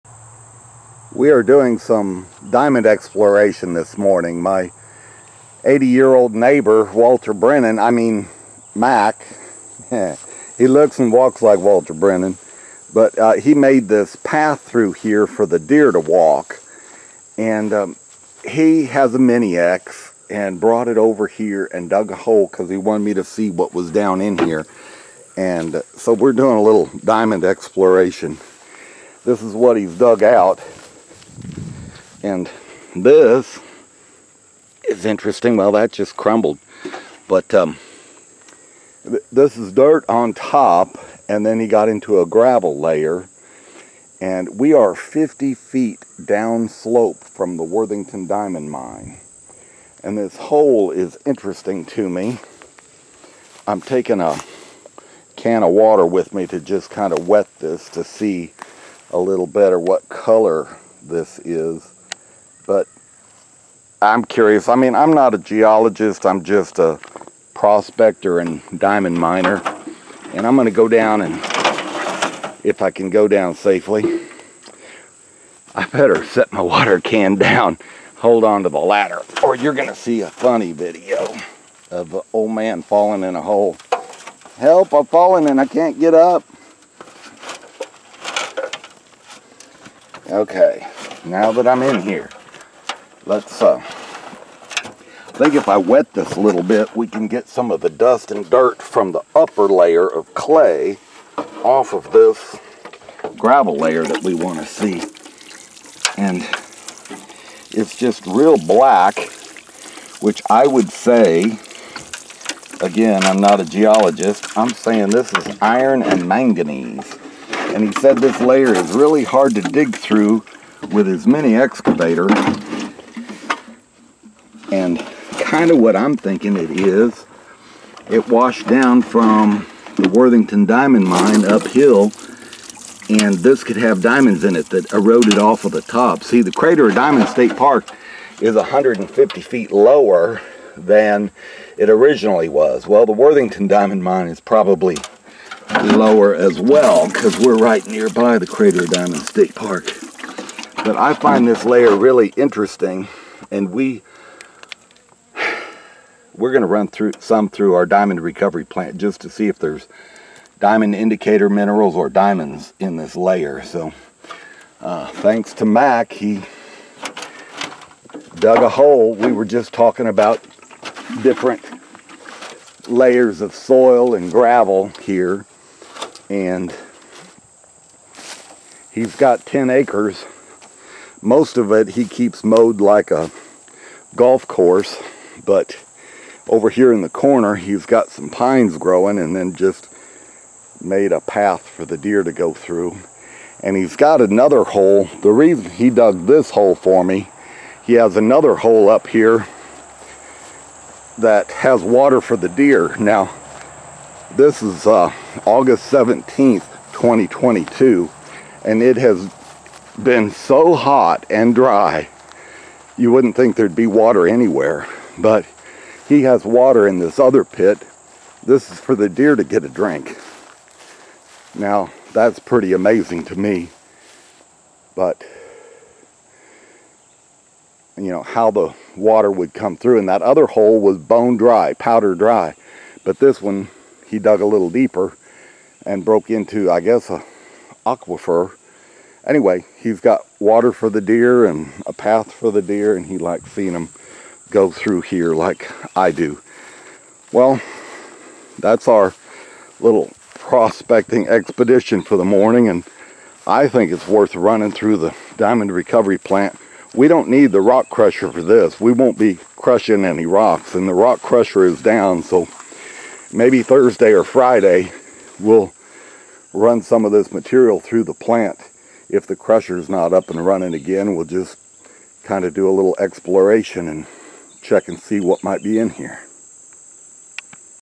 We Are Prospecting For Diamonds Today And Found A Promising Layer Of Gravel We Want To Sample
My neighbor used his mini-excavator to uncover and show me a layer of black gravel. It is about 2,000 feet from The Worthington Diamond Mine and 50 feet lower in elevation. Eons ago, erosion probably occurred there, some diamonds probably washed away, and they may have concentrated in this gravel layer. We plan to take a sample this week and run it through our diamond recovery plant. We will look for the three, main, diamond indicator minerals. and diamonds. Those indicator minerals are lustrous, black, chromium spinel, blood red or purple pyrope garnets, and grass green chromium diopside.